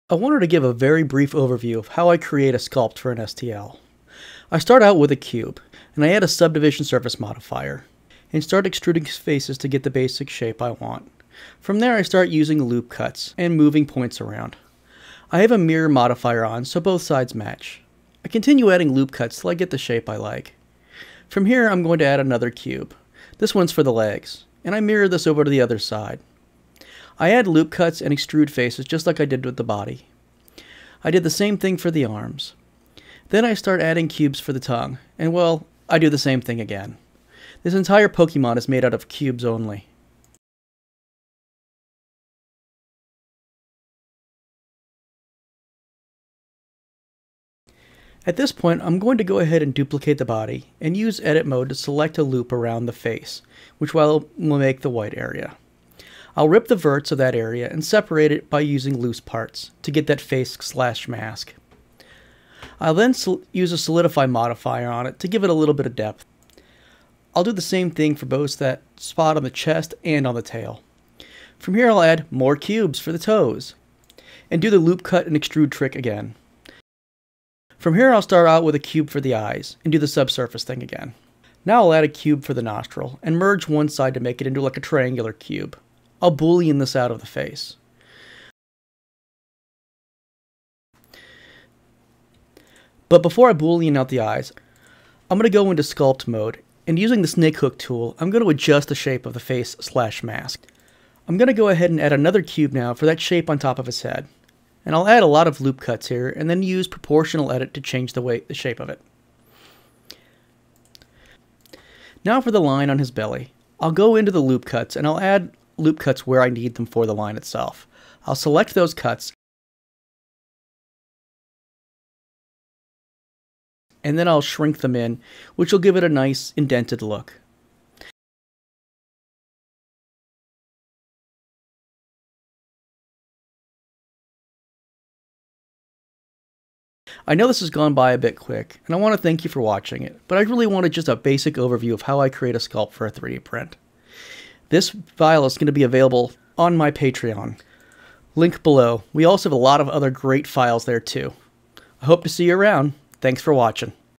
How to Sculpt Fuecoco in Blender
A Super quick guide on how to sculpt Fuecoco for 3d printing.